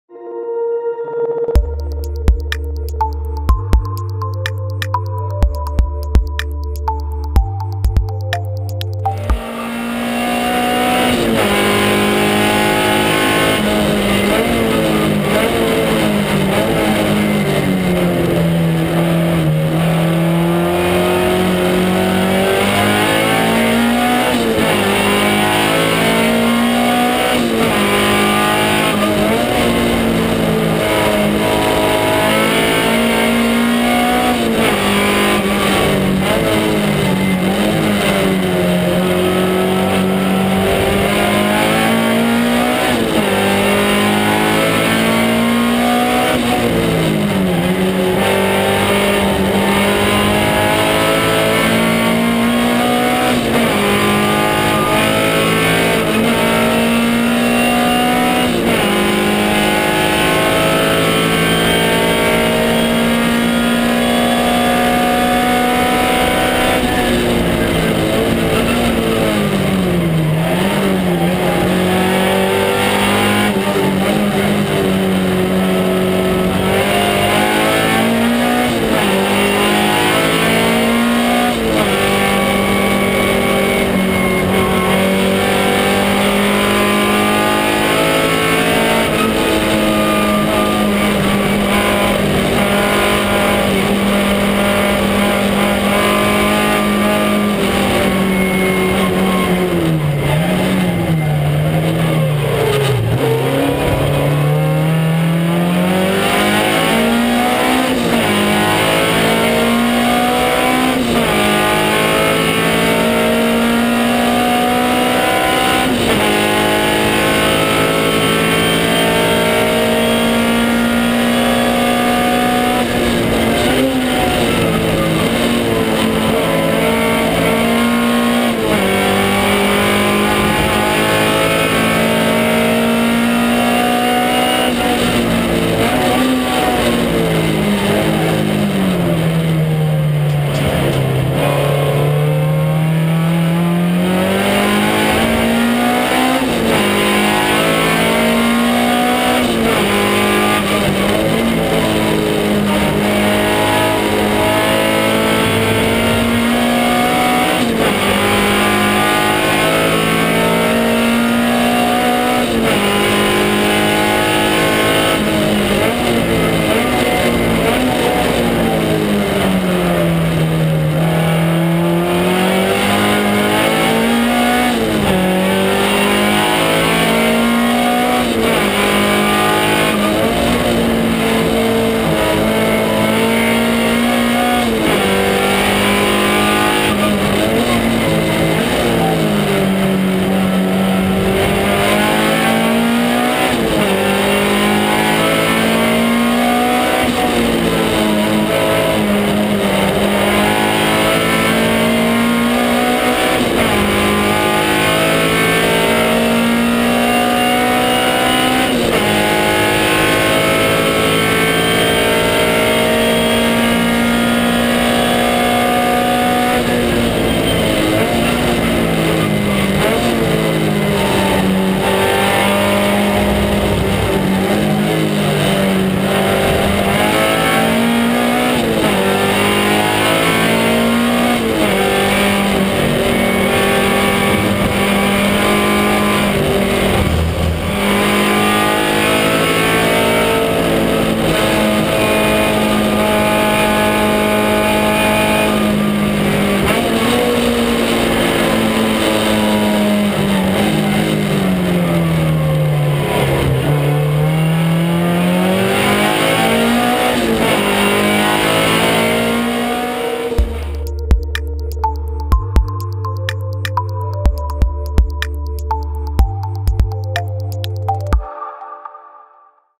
MSV Team Trophy Snetterton Season Starter 16/03/2013 Close racing.....
Just a couple of laps from team Big Skidz Ford Fiesta ST 1600 Cup Car. Unfortunately i had very little grip due to taking the wrong gamble on the weather and went for rain tyres instead of dry ones as i thought it was going to rain. Uniroyals are good but not good enough for the dry compared to Yoko A048r's.